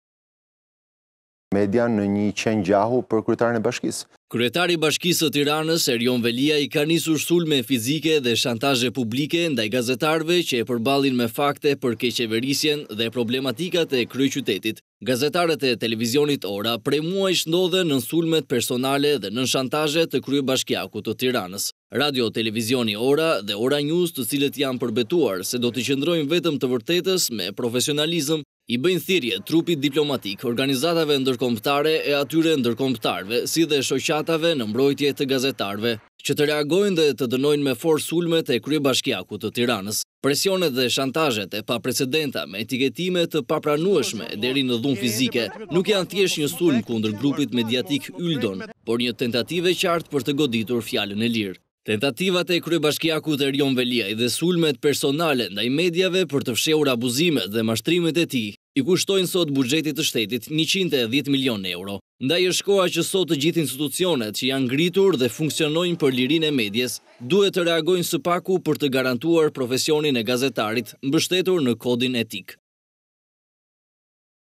Veliaj i quan gazetarët “qen”,Ora dhe Ora News thirrje diplomatëve të mbrojnë fjalën e lirë
Prej kohësh, Erion Veliaj vijon sulmet ndaj fjalës së lirë duke kërcënuar dhe shantazhuar gazetarët e Rtv Ora dhe Ora News, të cilët sjellin para ekranit të vërtetën e hidhur që hasen çdo ditë qytetarët, përtej fasadës së ofruar nga kryebashkiaku. Rtv Ora dhe Ora News i bën thirrje trupës diplomatike si edhe organizatave dhe shoqatave në mbrojtje të gazetarëve që të reagojnë dhe të dënojnë me forcë sulmet e Kryebashkiakut të Tiranës.

“E kthen median në një qen gjahu për kryetarin e Bashkisë”.

Kryetari i bashkisë së Tiranës, Erion Veliaj, ka nisur sulme fizike dhe shantazhe publike ndaj gazetarëve që e përballin me fakte për keqqeverisjen dhe problematikat e kryeqytetit.

Gazetarët e Televizionit Ora prej muajsh ndodhen nën sulmet personale dhe nën shantazhe të Kryebashkiakut të Tiranës.

Radio Televizioni Ora dhe Ora News, të cilët janë përbetuar se do ti qëndrojnë vetëm të vërtetës me profesionalizëm i bëjnë thirrje trupit diplomatik, organizatave kombëtare e atyre ndërkombëtarëve si dhe Shoqatave në Mbrojtje të Gazetarëve që të reagojnë dhe të dënojnë me forcë sulmet e Kryebashkiakut të Tiranës.

Presionet dhe shantazhet e paprecedenta me etiketime të papranueshme e deri në dhunë fizike nuk janë thjesht një sulm kundër Grupit Mediatik Yldon por një tentativë e qartë për të goditur fjalën e lirë.

Tentativa e kryebashkiakut Erion Veliaj dhe sulmet personale ndaj mediave për të fshehur abuzimet dhe mashtimet e tij i kushtojnë sot buxhetit të shtetit 110 milionë euro.

Ndaj është koha që sot të gjithë institucionet që janë ngritur dhe funksionojnë për lirinë e medias, duhet të reagojnë së paku për të garantuar profesionin e gazetarit mbështetur në kodin etik.